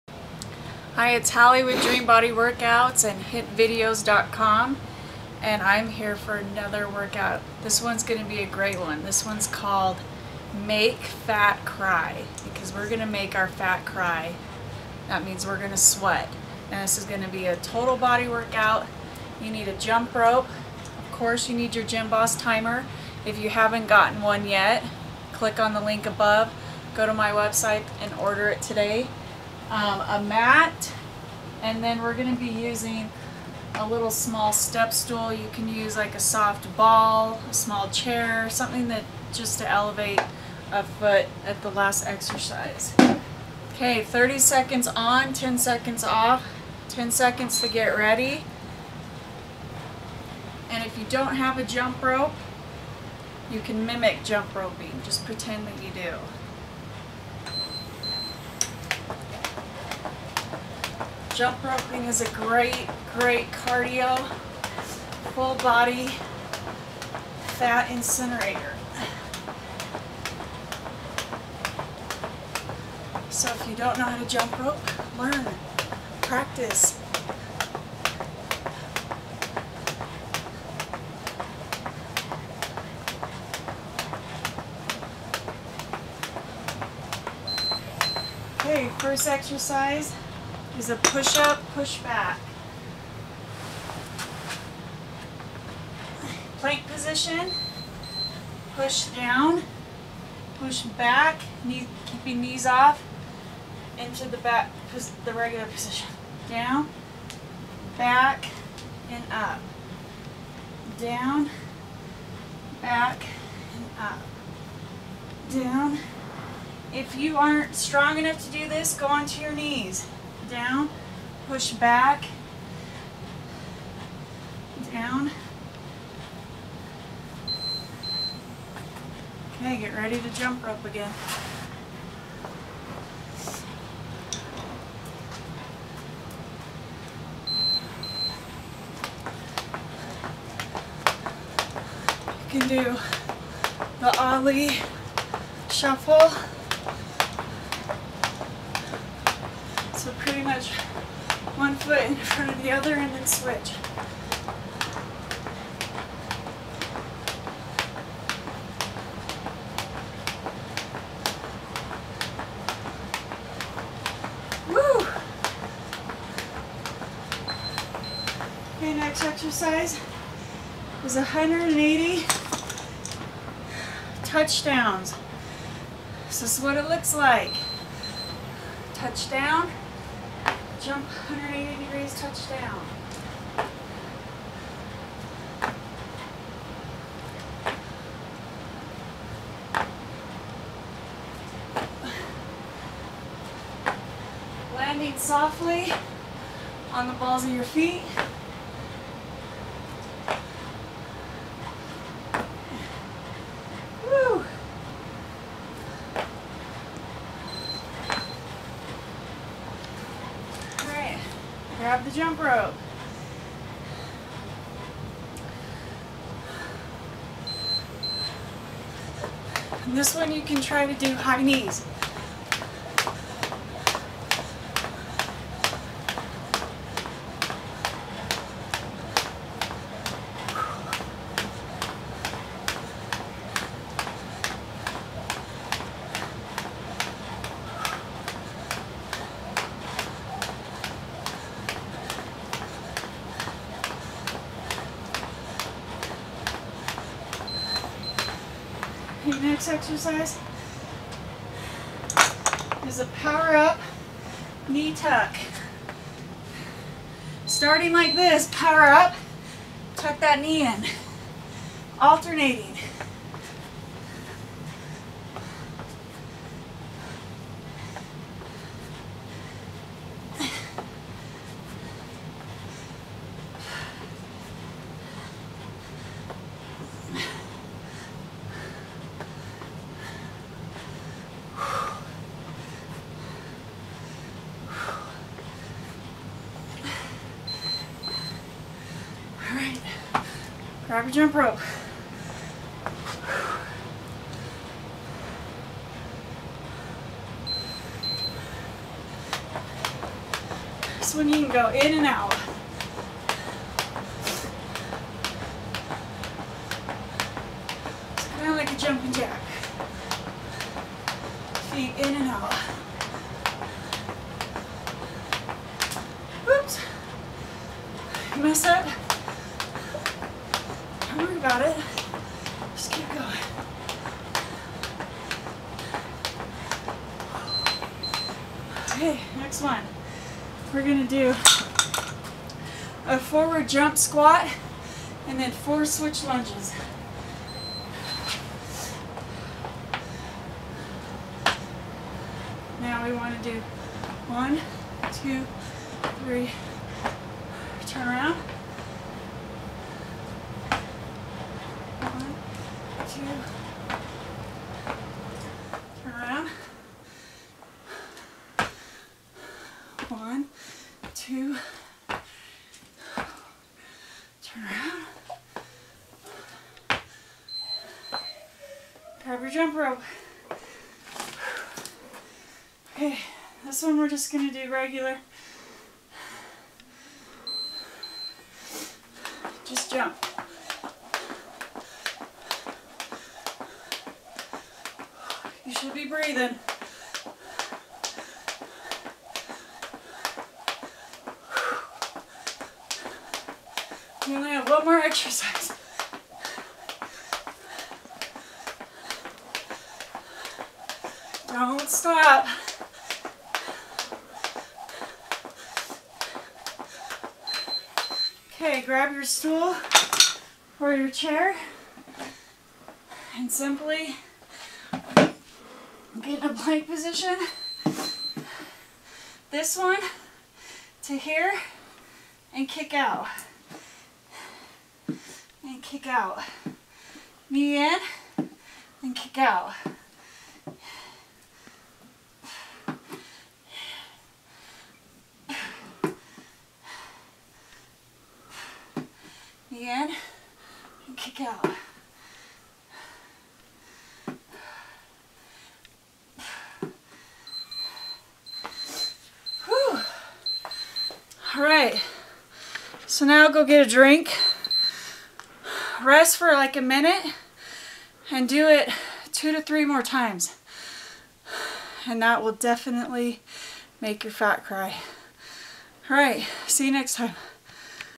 Make FAT Cry!
Show Support & HIIT "Like" for Make Fat Cry!

This Workout..Make Fat Cry! Will make you Sweat , Burn Fat and Build Lean Muscle...It is a Full Body Workout. You will need a Gymboss Timer, Jump Rope, Mat and a small stool or ball...You will do 10 exercises for 30 seconds on and 10 seconds off.. #1 Jump Rope #2 Push Back Push-up #3 Jump Rope #4 180* Touchdowns #5 Jump Rope #6 Power-up Knee Tucks #7 Jump Rope #8 Forward Jump Squat then 4 Jump Lunges #9 Jump Rope #10 Knee to elbow then leg out to side then alt... Perform this 2-3 times with a minute break in between...Push yourself Hard and results will come!